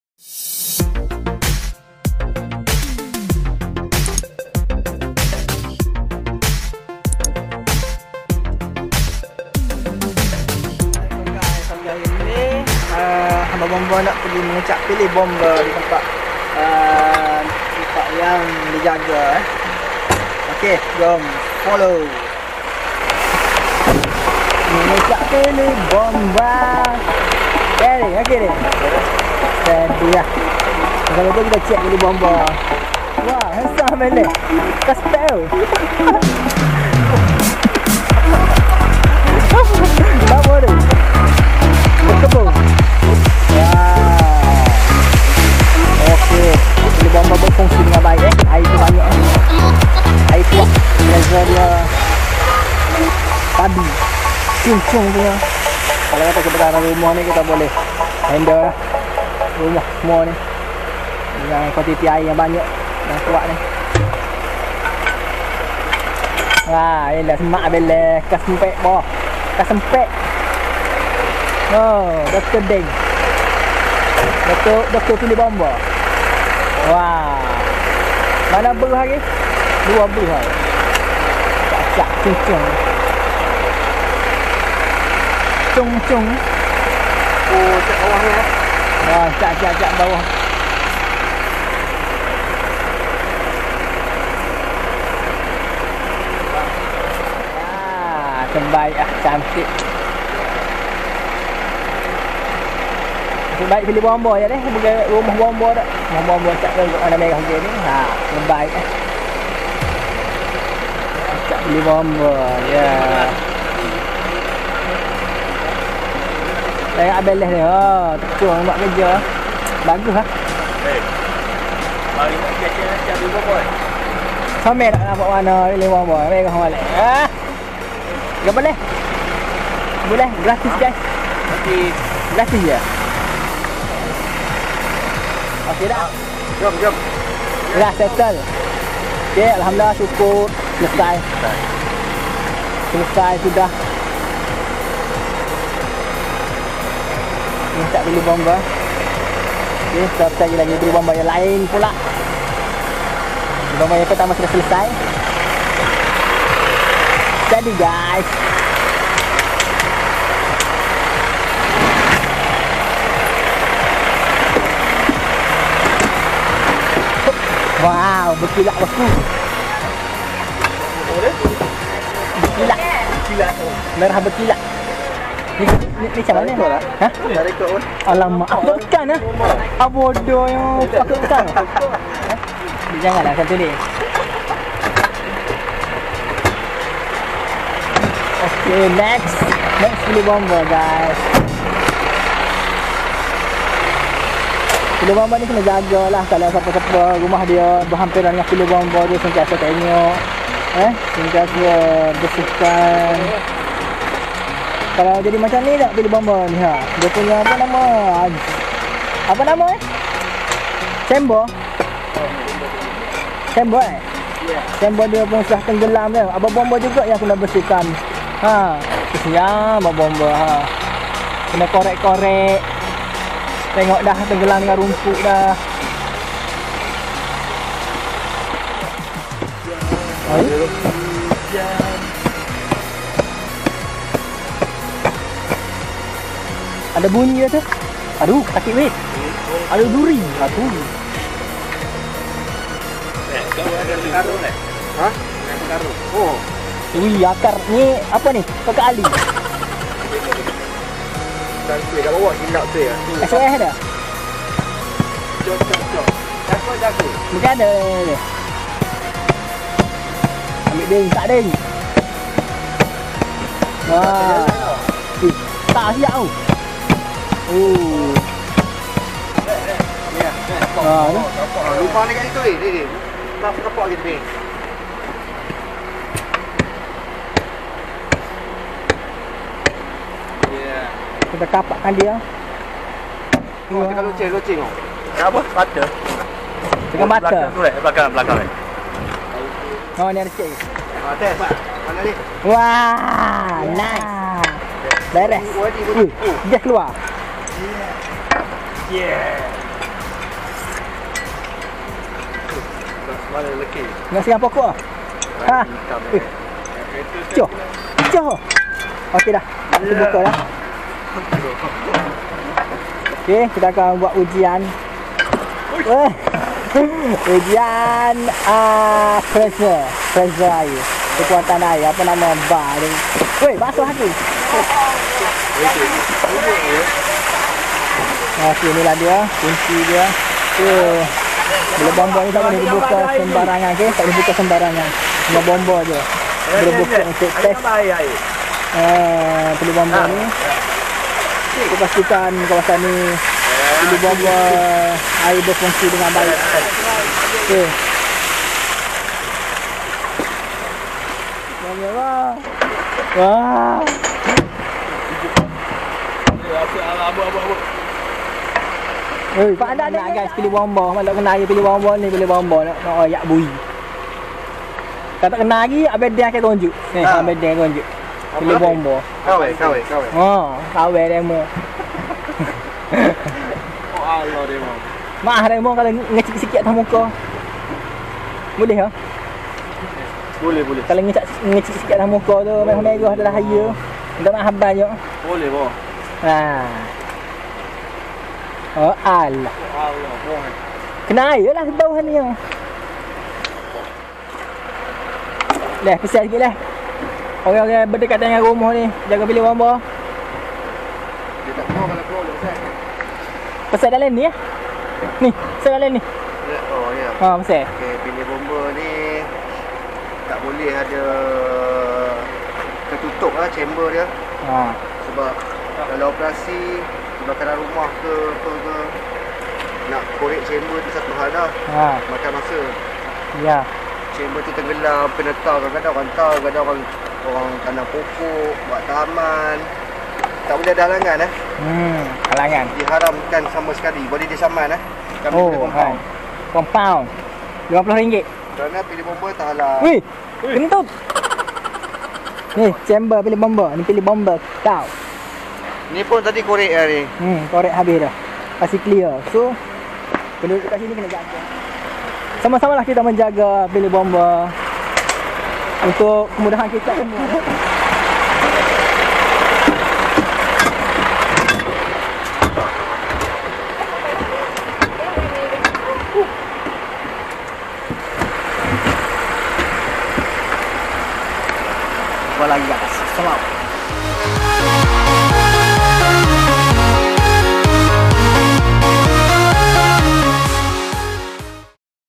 KEPENTINGAN PILI BOMBA DI KAWASAN RUMAH ANDA SEMUA
pihak bomba membuat pemeriksaan dan mengecat pili bomba dalam kawasan jagaan